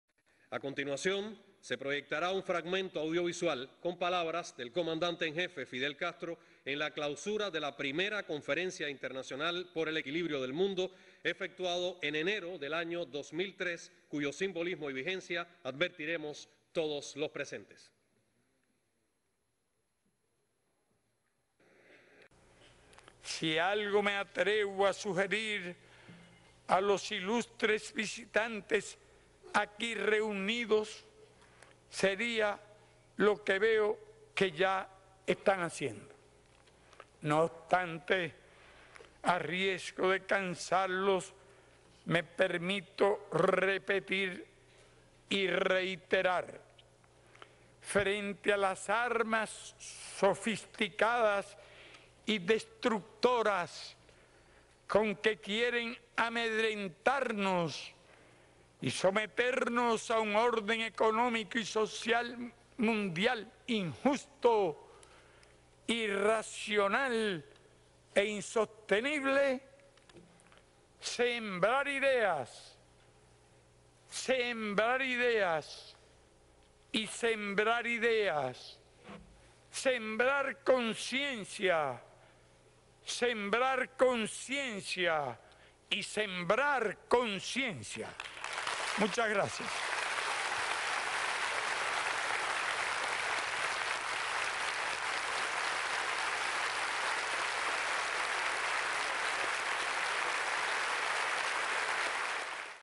Fidel siempre presente_ IV Conferencia Internacional POR EL EQUILIBRIO DEL MUNDO, enero 2019
Clip de video proyectado en la inauguración de la IV Conferencia Internacional POR EL EQUILIBRIO DEL MUNDO el 28 de enero de 2019. Contiene un emotivo fragmento del discurso que Fidel pronunciara a propósito de la clausura de a I Conferencia Internacional POR EL EQUILIBRIO DEL MUNDO, enero de 2003:

“Si algo me atrevo a sugerir a los ilustres visitantes aquí reunidos, sería lo que veo que ya están haciendo. No obstante, a riesgo de cansarlos, me permito repetir y reiterar: frente a las armas sofisticadas y destructoras con que quieren amedrentarnos y someternos a un orden económico y social mundial injusto: irracional e insostenible: ¡sembrar ideas, sembrar ideas y sembrar ideas!; ¡sembrar conciencia, sembrar conciencia, y sembrar conciencia! ¡Muchas gracias!”